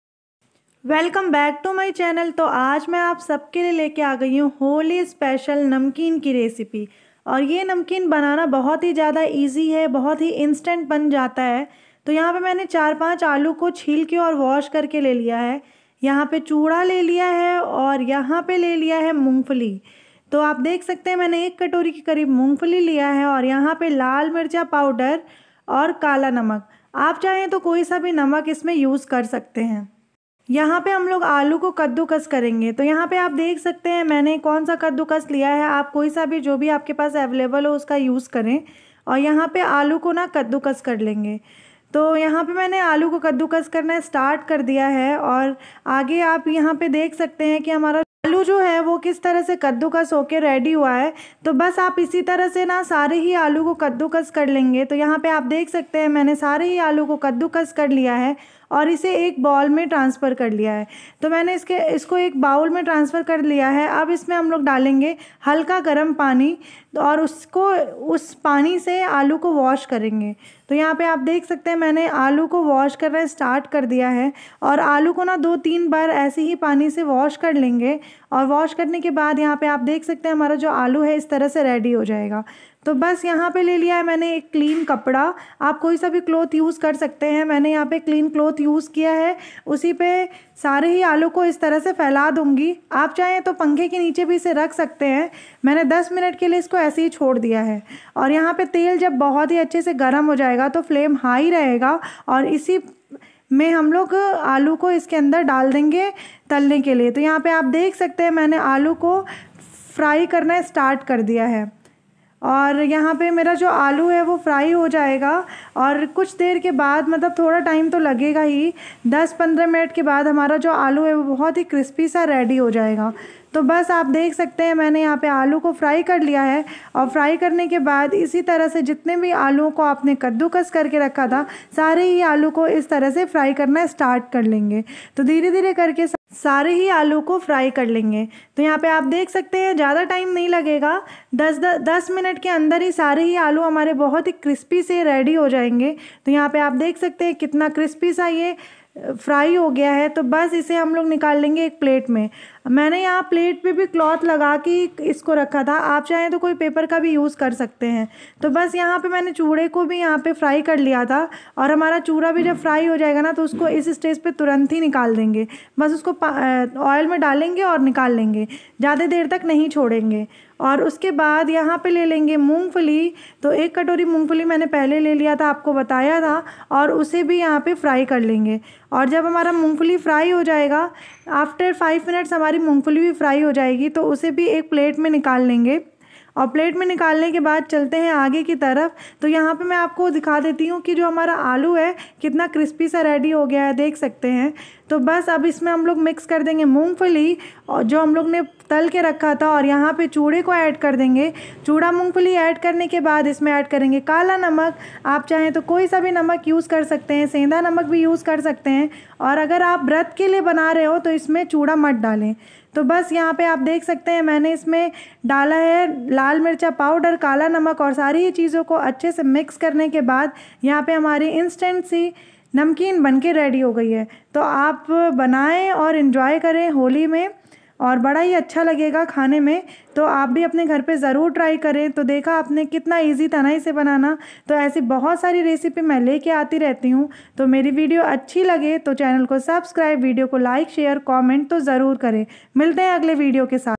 15 मिनट में आलू की झटपट नमकीन तैयार हो जाएगी और तेल भी ज्यादा नहीं लगेगा।
15 मिनट में आलू की झटपट नमकीन तैयार हो जाएगी और तेल भी ज्यादा नहीं लगेगा।

aaj tanuksha ke kitchen mein banaa rahe Hain ham aalu ki instant namkin Holi ke liye..
aap bhi zrur try kre

ingredients
aalu
chuda
mungfali
oil
namak
Lal mircha powder